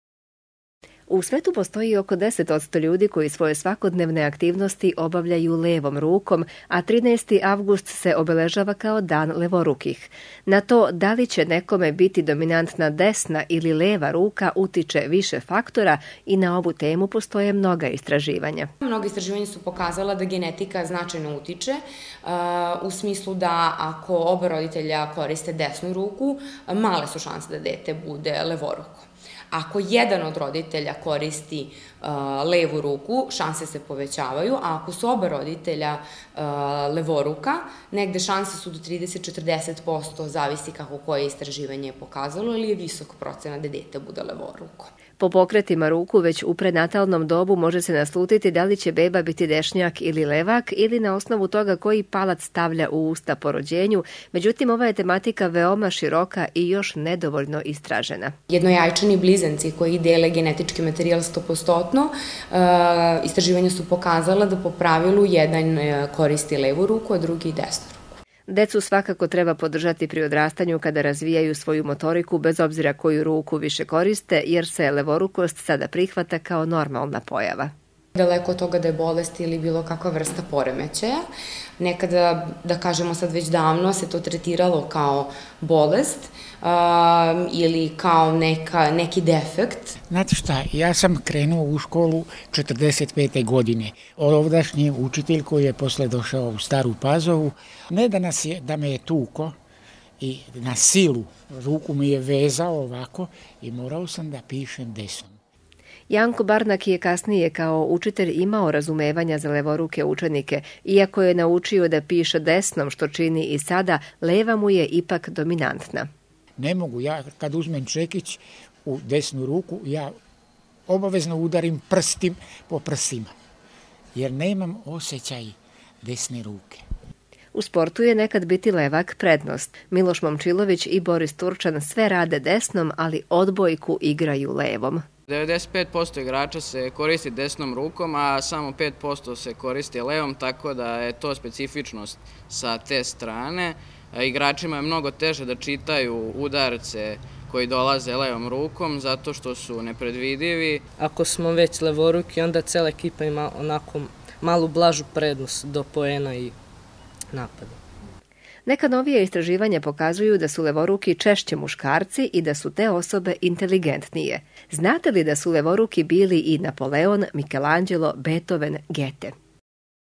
Biti levoruk prednost ili mana
U susret Danu levorukih, koji se obeležava 13. avgusta istraživali smo zašto neki ljudi više koriste levu ruku, i da li je to prednost ili mana?